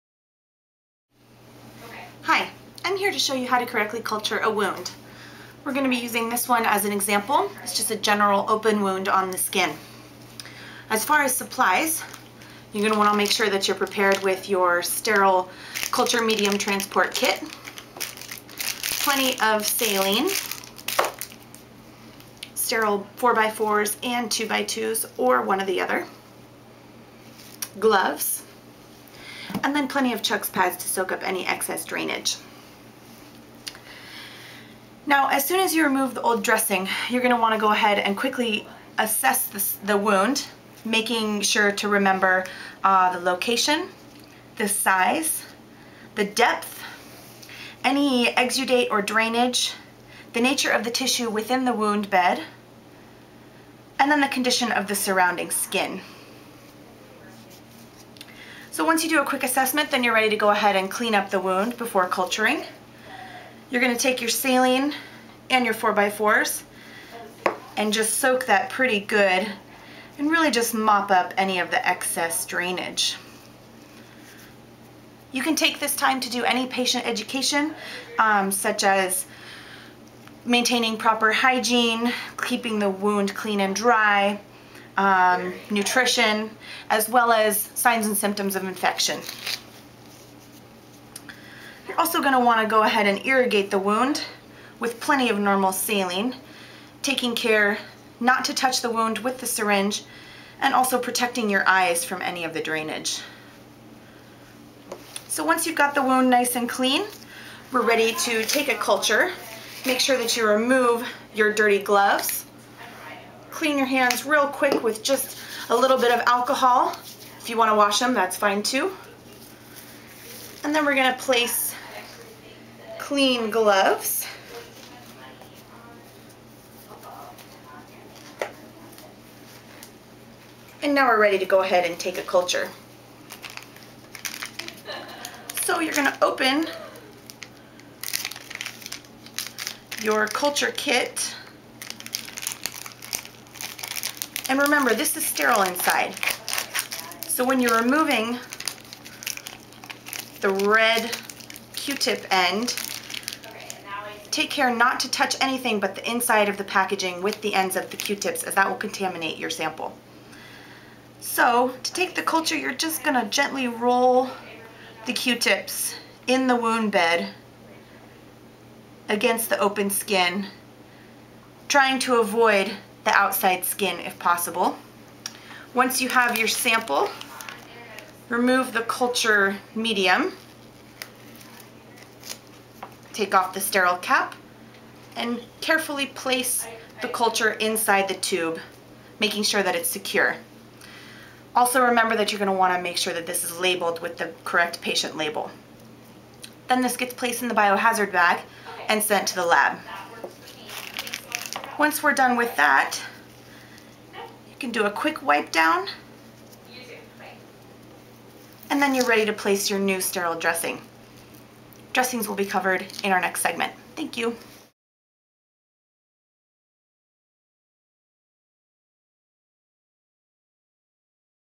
Obtaining a Wound Culture
Learn how to correctly cleanse and obtain a wound culture before applying a new dressing